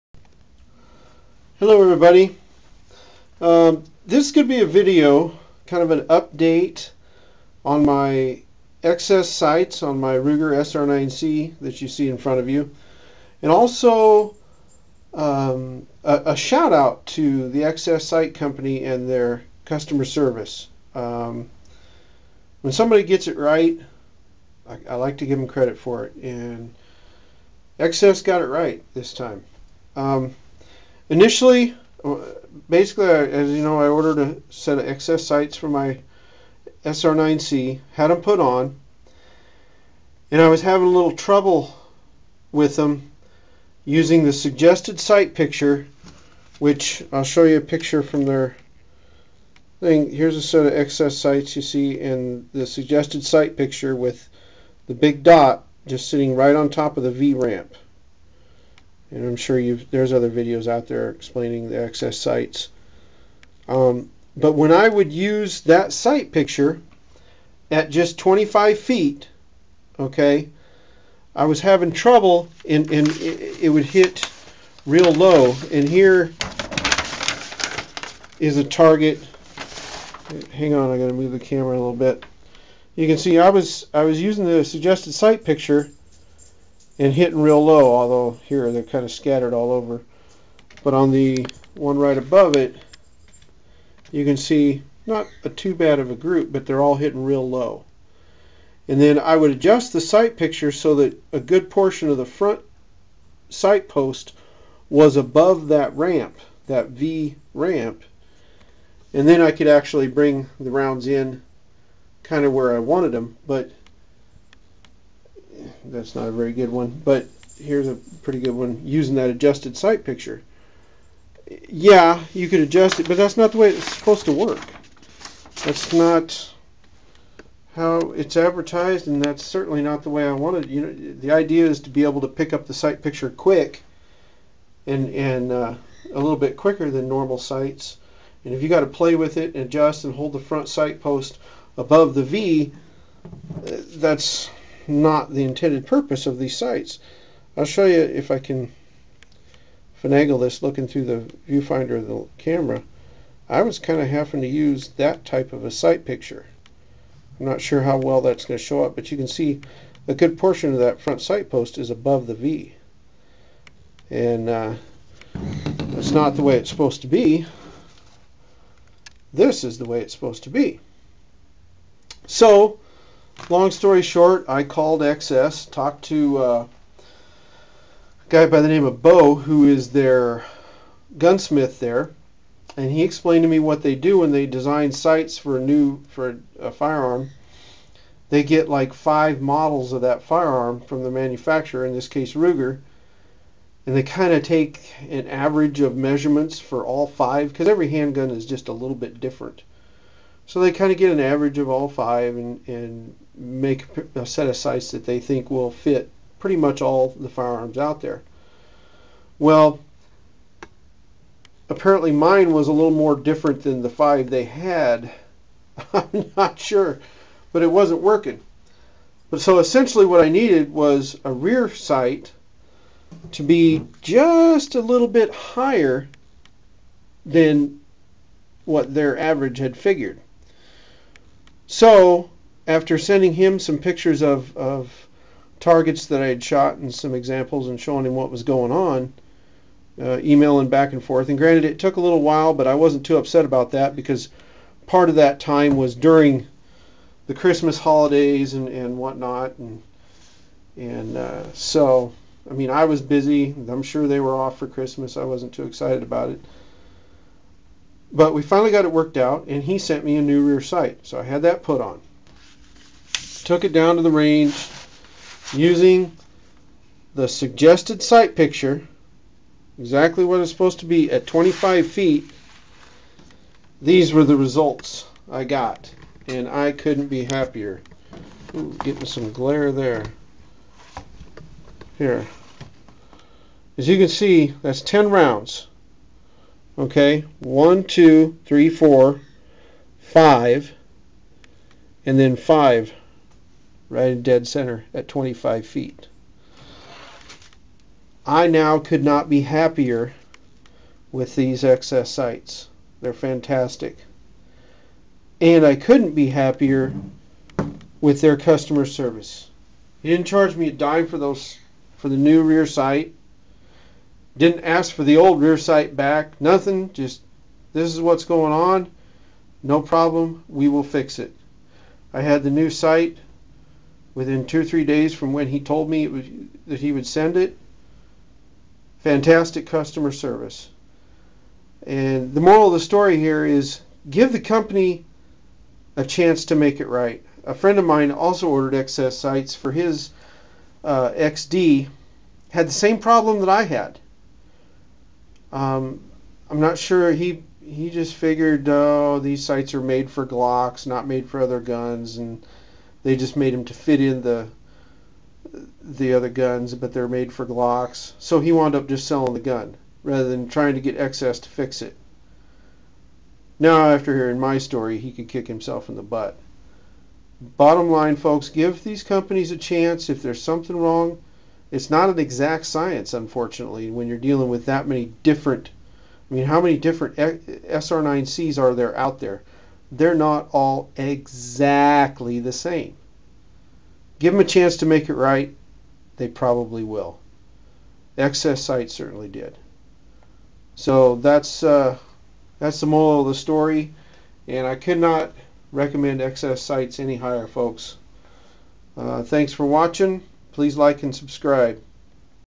XS Sights customer service
I was having problems with my XS Sights. Shooting low. Talked with XS and they sent new rear sight. Problem sloved. Good job XS. Very nice customer service.